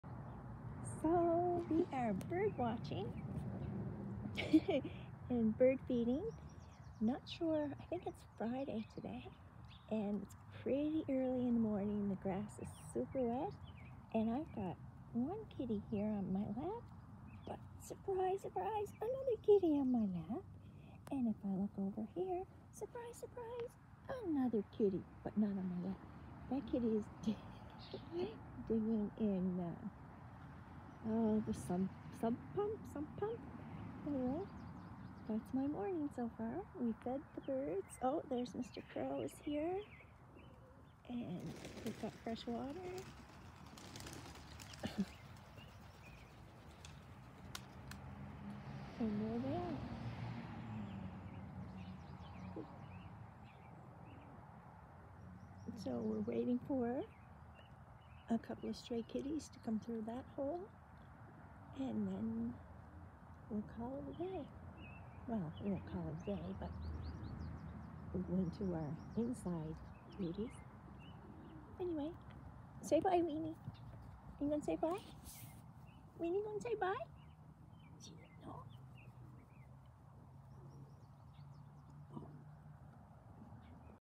Scruffy cats need cuddles
Rainy day wet snuggles. At least there’s no wet dog smell 🤣